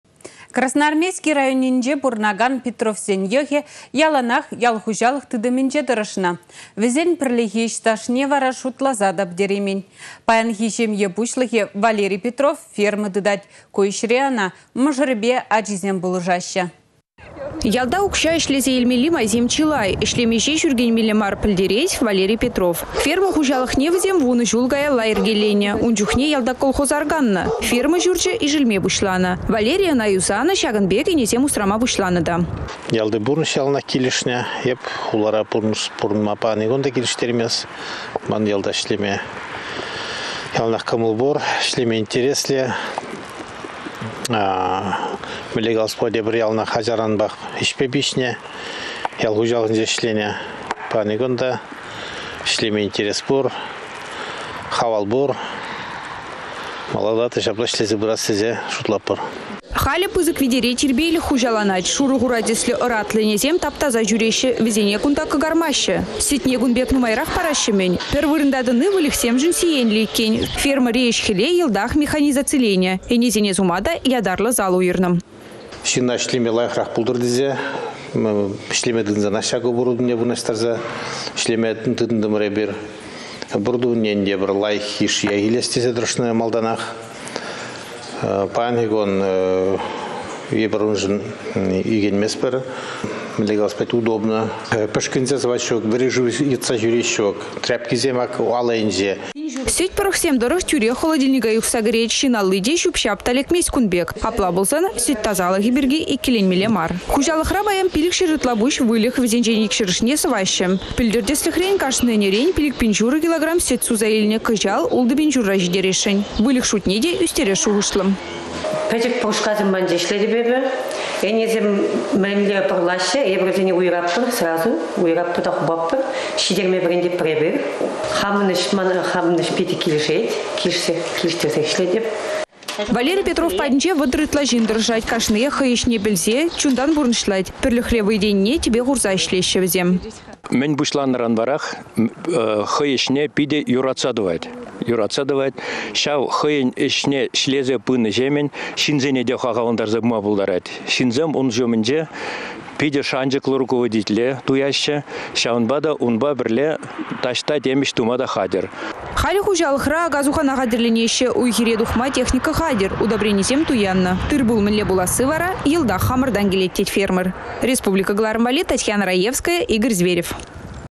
Ачаранпах выльăхсене юратнă
Красноармейски районĕнче пурăнакан  Петровсен йăхĕ яланах ял хуçалăх тытăмĕнче тăрăшнă. Вĕсен пĕрлехи ĕç стажне вара  шутласа та пĕтереймĕн. Паянхи çемье пуçлăхĕ Валерий Петров ферма тытать. Ку ĕçре ăна мăшăрĕпе ачисем  пулăшаççĕ.